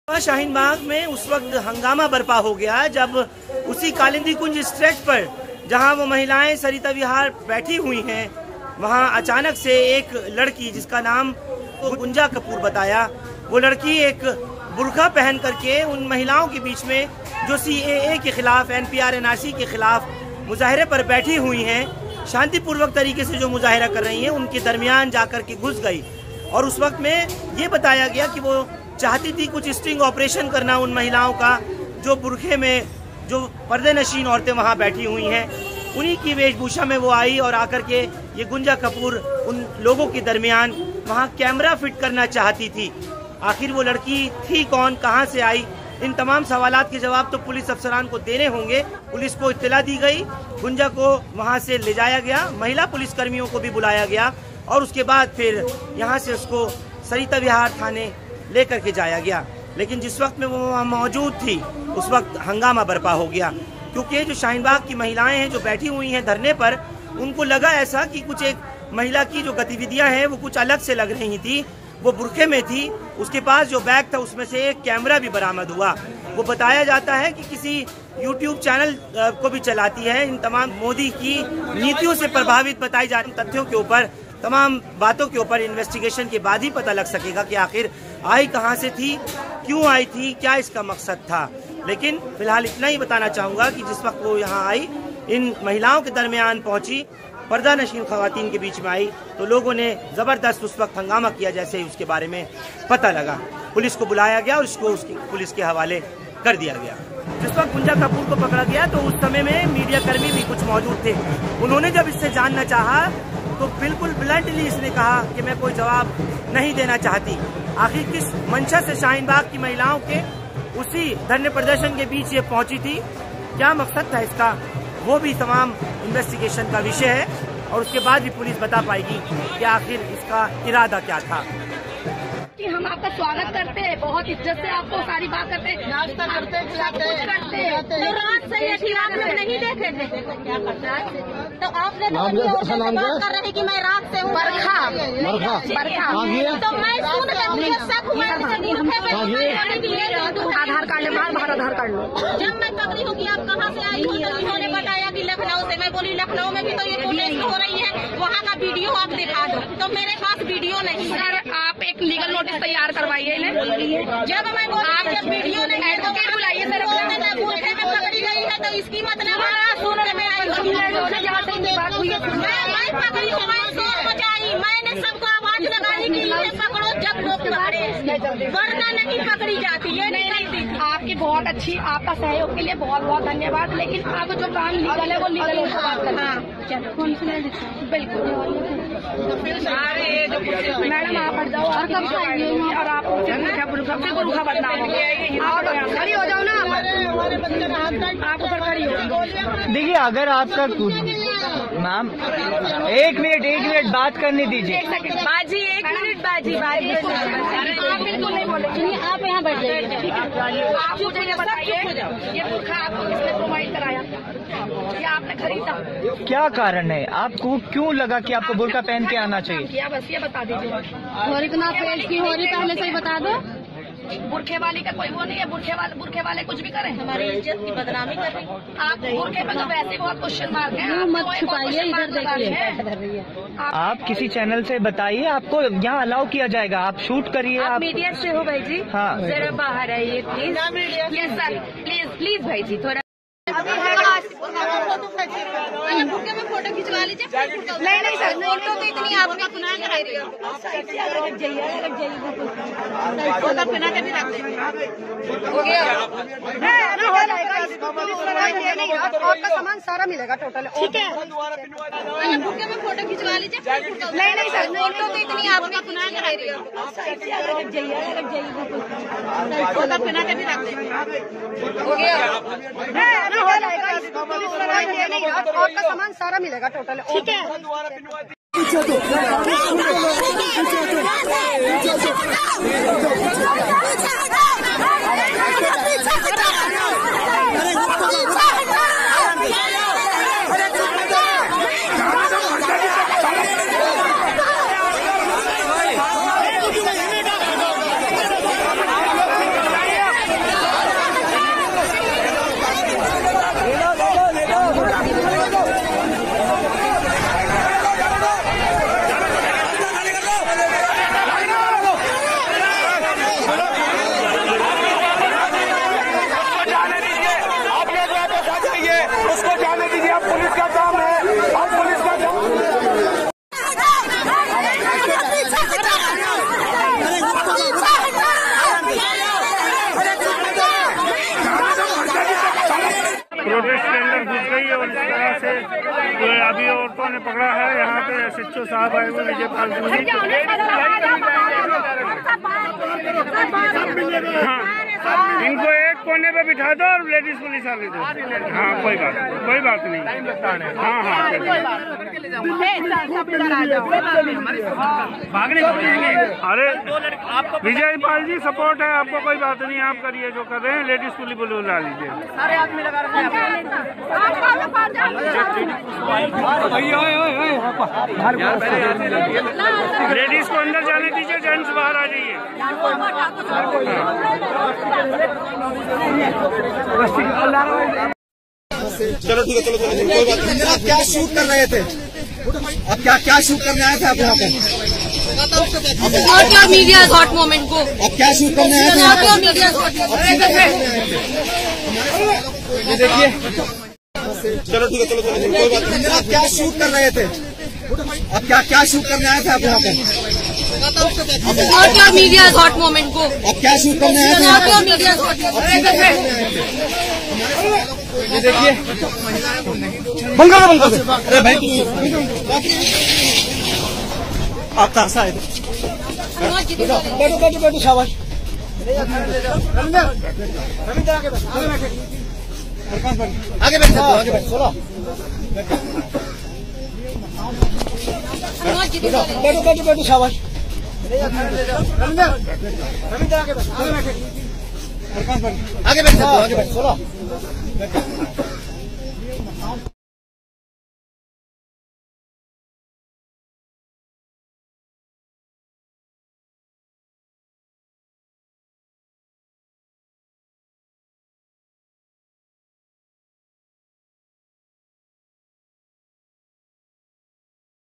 बुर्क़ा पहन,रूप बदल कर#शाहीनबाग़ की माँ बहनों के बीच से पकड़ी गई गुँजाकपूर GunjaKapur कैमरे के साथ
बुर्क़ा पहन,रूप बदल कर#शाहीनबाग़ की माँ बहनों के बीच ख़ुफ़िया कैमरे के साथ गुँजाकपूर पकड़ी गई#GunjaKapur